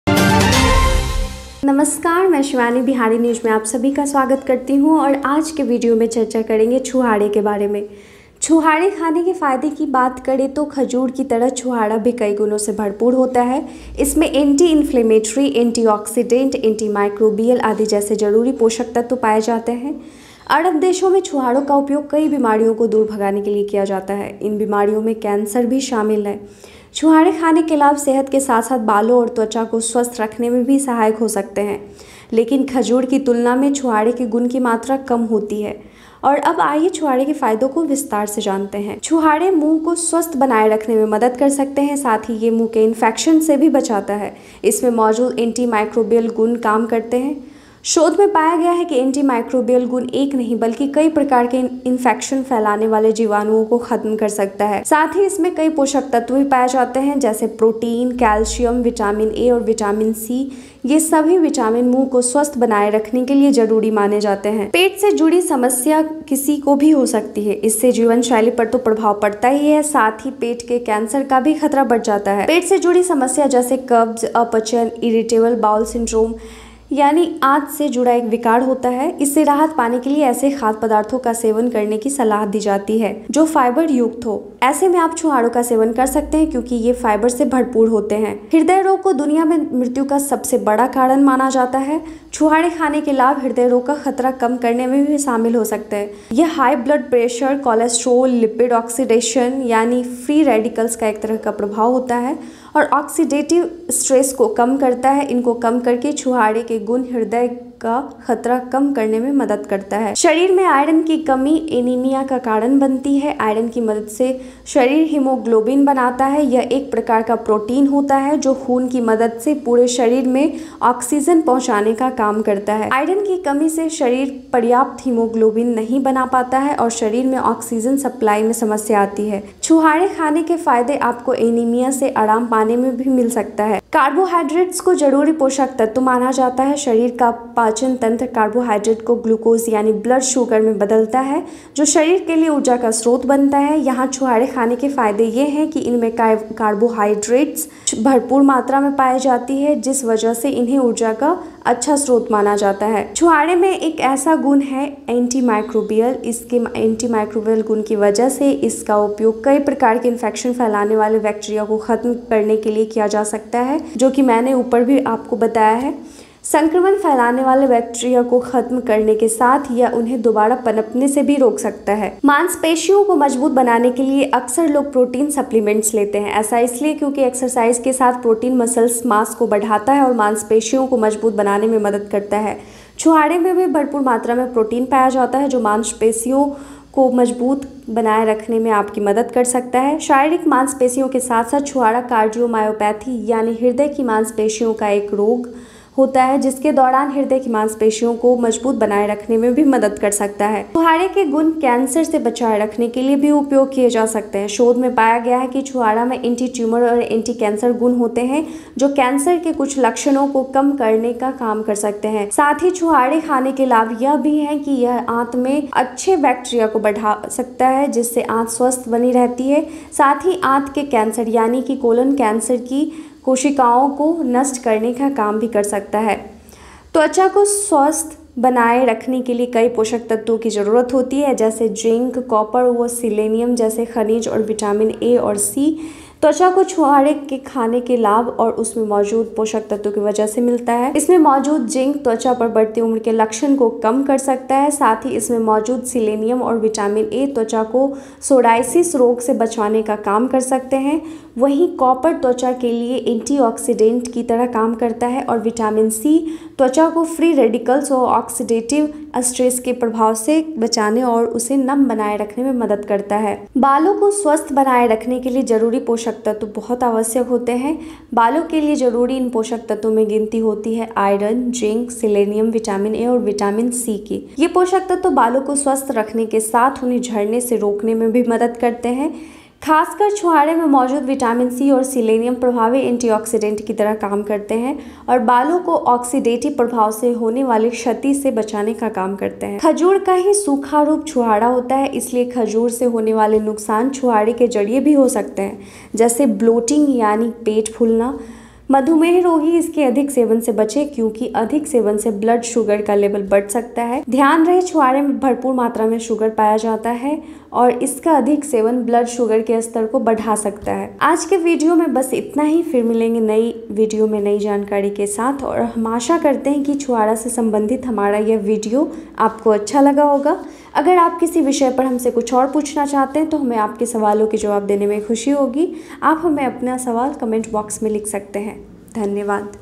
छुहारे के फायदे.Benefits of Dry dates in diebetes, pregnancy, heart,weight loss,weight gain.
Dates can help to keep the mouth healthy. They also protect against mouth infection. The antimicrobial properties present in it work. Research has found that antimicrobial properties can eliminate bacteria in not one but several types of infection spreaders. Also, many nutrients are found in it, such as protein, calcium, vitamin A and vitamin-C. All these vitamins are considered essential for keeping the mouth healthy.

 Anyone can have stomach related problems. This not only affects the lifestyle, but also increases the risk of stomach cancer. In order to get relief from stomach problems like constipation, indigestion, irritable bowel syndrome i.e. intestinal disorders etc., it is recommended to eat foods that are fiber rich. In such a situation, you can take the dates, because they are full of fiber

 Heart disease is considered the biggest cause of death in the world. The benefits of eating dates can also be found in reducing the risk of heart disease. It reduces high blood pressure, cholesterol, lipid oxidation ie a kind of effect of free radicals and oxidative stress, by reducing them, the healing properties of the date date help reduce the risk of heart disease

 Iron deficiency in the body causes anemia. With the help of iron, the body makes hemoglobin. It is a type of protein, which works with the help of blood to deliver oxygen throughout the body. Due to lack of iron, the body cannot make enough hemoglobin and there is a problem in oxygen supply in the body. You can also get the benefits of eating date palm to get relief from anemia.

 Carbohydrates are considered essential nutrients. The digestive system of the body converts carbohydrates into glucose i.e. blood sugar, which becomes a source of energy for the body. The benefits of eating dates here are that they are rich in carbohydrates, due to which they are considered a good source of energy.

 One such quality is antimicrobial in date palm. Because of its antimicrobial property, it can be used to eliminate many types of infection-causing bacteria. Along with eliminating the bacteria that spread the infection, it can also prevent them from growing again.

 People often take protein supplements to strengthen muscles. This is because protein exercise along with exercise increases muscle mass and helps in strengthening the muscles. Dates are also rich in protein, which can help you in maintaining strong muscles. Along with the physical muscles, the date can also help to keep the heart muscle strong during the period of cardiomyopathy i.e. heart muscle disease.

 The date-of-date properties can also be used to protect against cancer. Research has found that the date date has antitumor and anti-cancer properties, which may work to reduce some symptoms of cancer. Also, the benefits of eating dates are also that it can increase the good bacteria in the intestine, which keeps the intestines healthy. Also, it can also act to destroy the cells of colon cancer ie colon cancer.

 Many nutrients are needed to keep the skin healthy. For example, minerals like zinc, copper and selenium and vitamins A and C. The skin can get the benefits of eating date palm due to the nutrients present in it. The zinc present in it can reduce the signs of aging on the skin

 Also, the selenium and vitamin A present in it can protect the skin from psoriasis disease. At the same time, copper reduces antioxidants for the skin and Vitamin-C helps protect the skin from the effects of free radicals and oxidative stress and keep it moist.

The nutrients required to keep hair healthy are very important. These nutrients are counted as essential for hair, iron, zinc, selenium, vitamin A and vitamin C. These nutrients keep the hair healthy and also help prevent them from falling out. In particular, the vitamin C and Silenium present in the date date act as effective antioxidants and protect the hair from the damage caused by oxidative effects.

The dry form of date palm is dateless. Therefore, dates can also be harmed by date palm. like
Bloating means flatulence
Diabetics should avoid excessive intake, as higher intake can increase blood sugar levels.
Keep in mind that sugar is found in plenty in date palm and its high intake can increase the level of blood sugar.